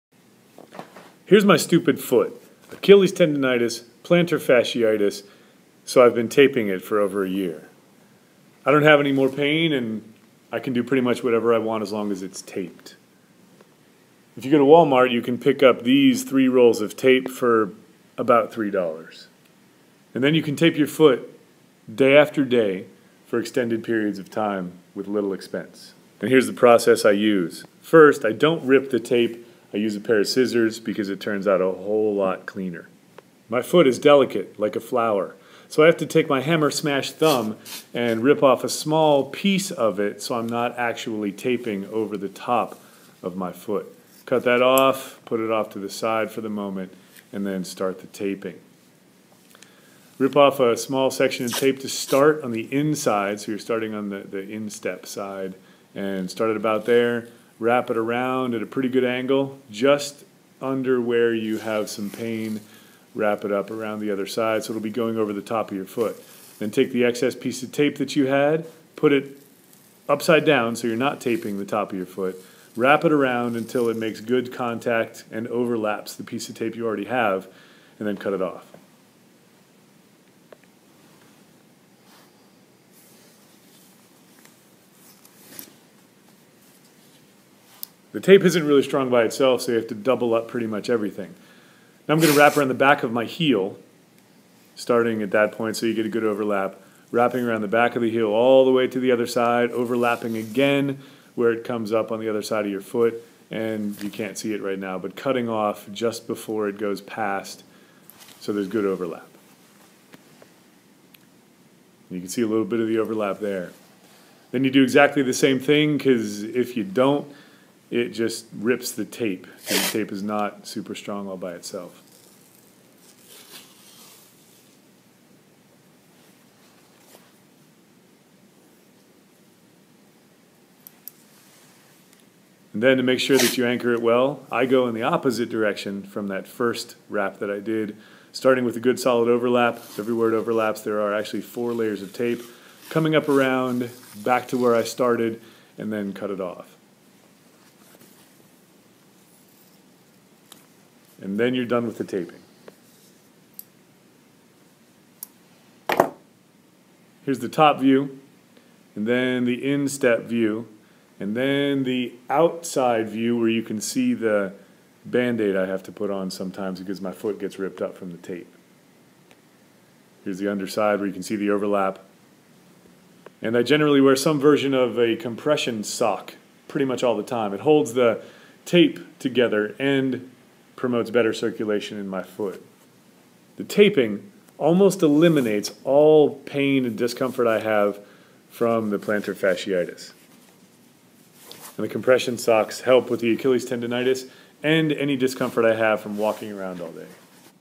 Foot Tape for Plantar Fasciitis - Problem Solved!
Thanks for LIKING - How to tape your foot to minimize pain and encourage healing. I've tried many taping methods and this is the best I've found. I don't stretch nor do I do any foot specific exercises. Two or three minutes every morning and the problem's gone.Cortisone shots did NOT help.  A boot did NOT help. Taping it allows me to go through the day with minimal pain.  Now I can take regular walks and that allows my foot to get stronger and then I can quit taping it.